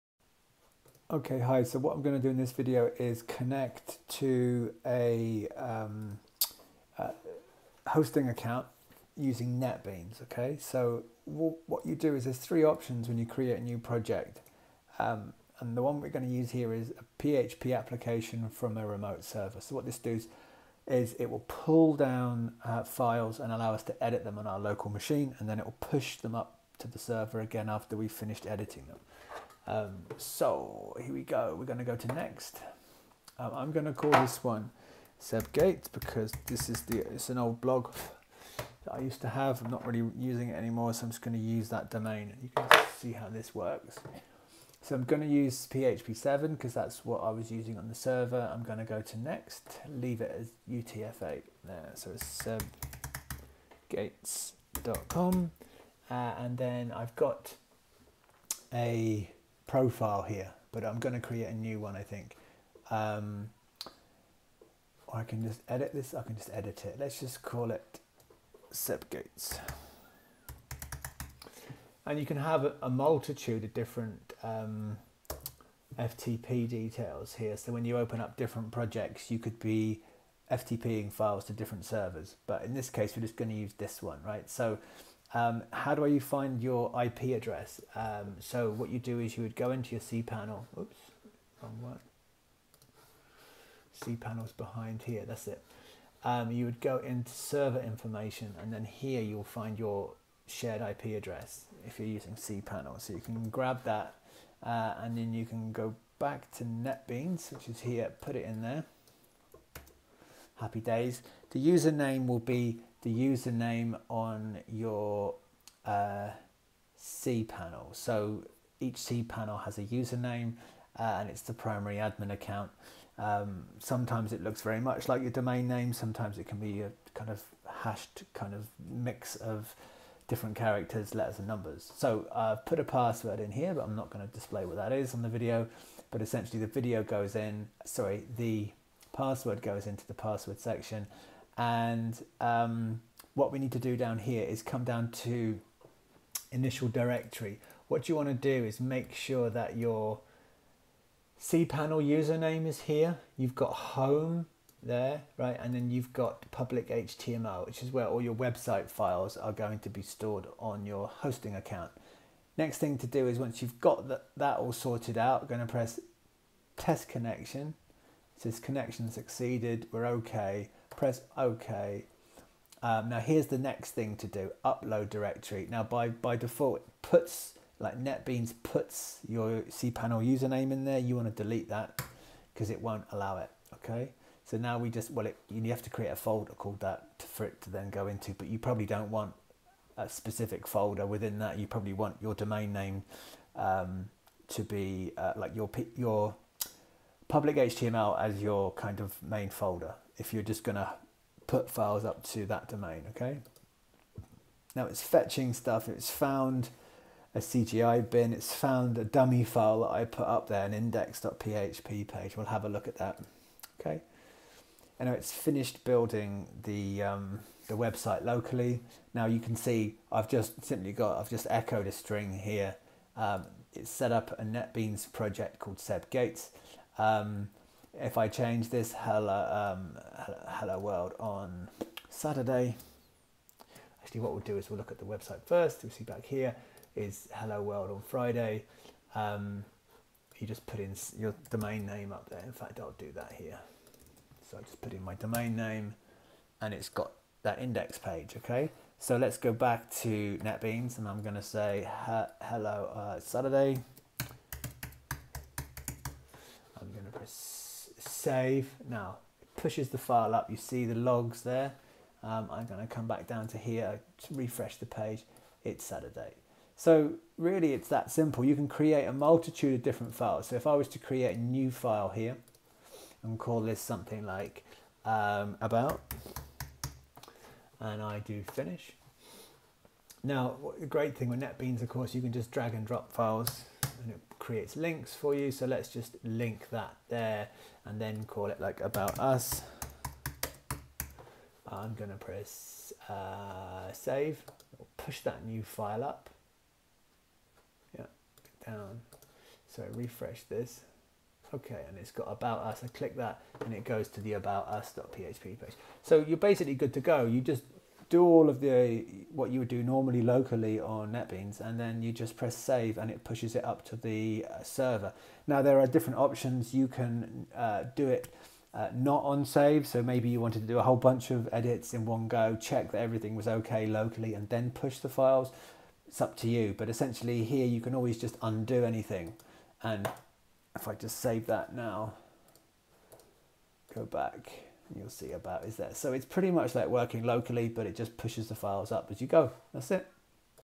How to FTP files to a remote server using Netbeans for MAC
In this tutorial I show you How to FTP PHP files to a remote server using Netbeans for MAC. The process is very simple and makes pushing changes to a PHP website a breeze. I will be demonstrating how to deploy a project to a Linux Control Panel.